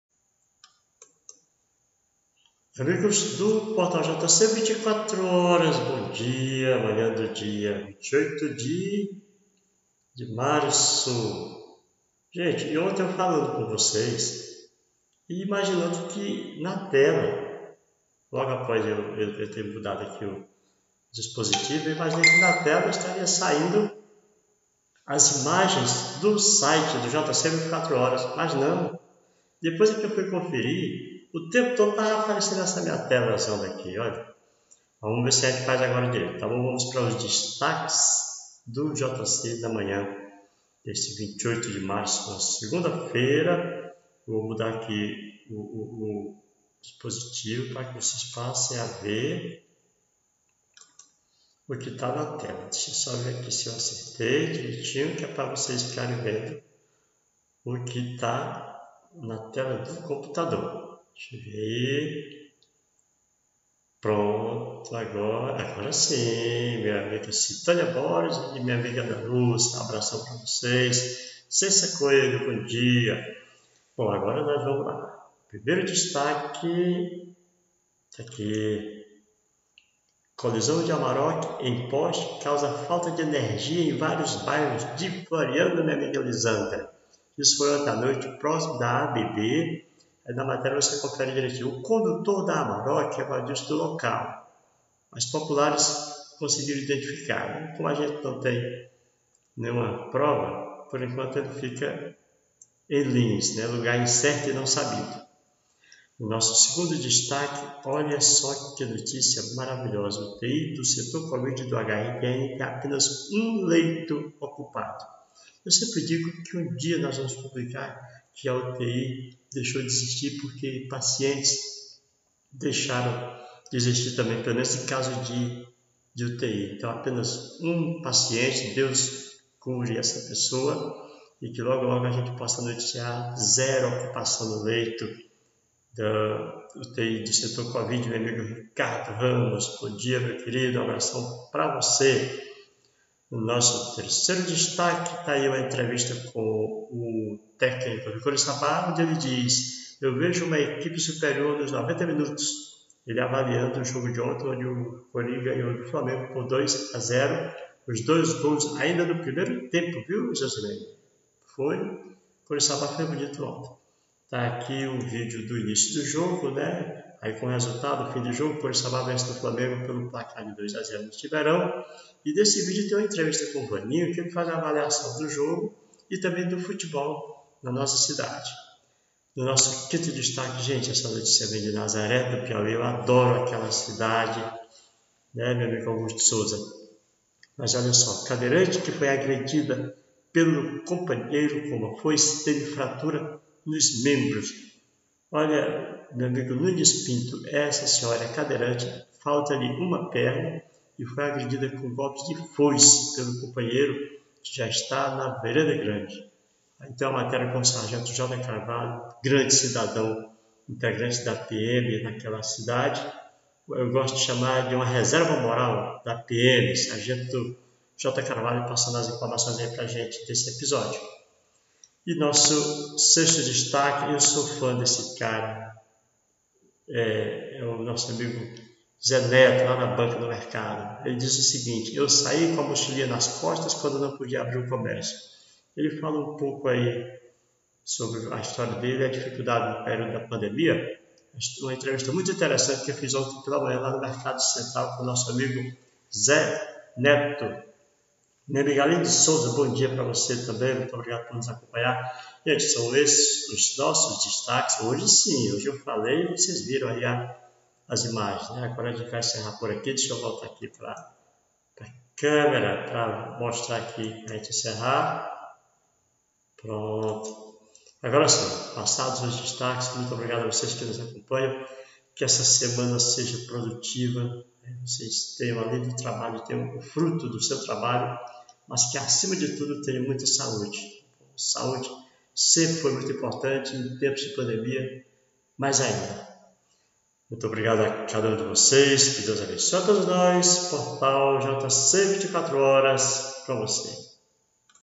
Confira os destaques do jc24horas neste dia 28/03/2022
você confere as matérias que nós anunciamos neste vídeo.  O acidente da Amarok que bateu no poste perto da AABB, os números do setor covid do HRTN, uma entrevista com o Toninho pesso, Técnico do Corissabbá, o caso da cadeirante que foi agredida com golpes de foice pelo companheiro e finalmente uma entrevista com o Zé Neto, comerciante do mercado central. Tudo isso e muito mais você confere no